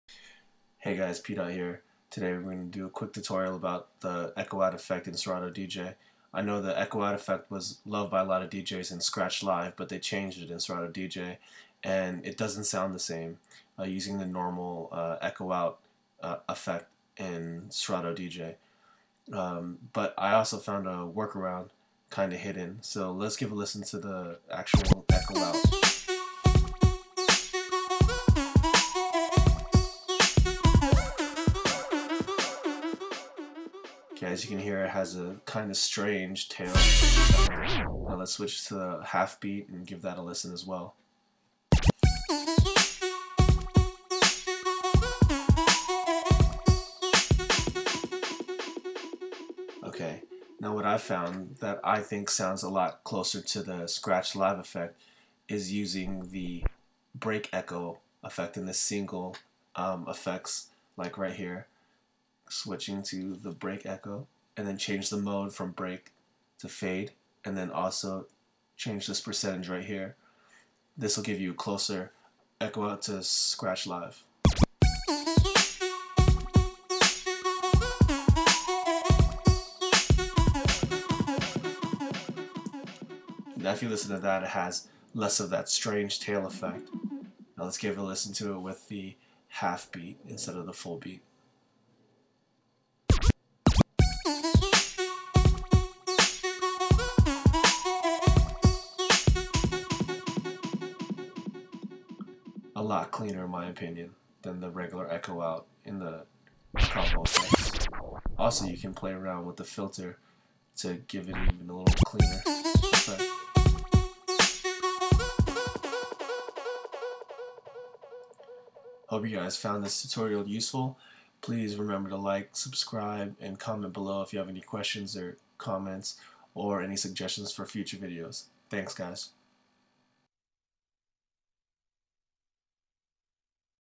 Serato DJ - How To Use Hidden Echo Out Effect
This version of the echo out effect sounds a lot cleaner than the echo out combo effect that is listed in Serato DJ.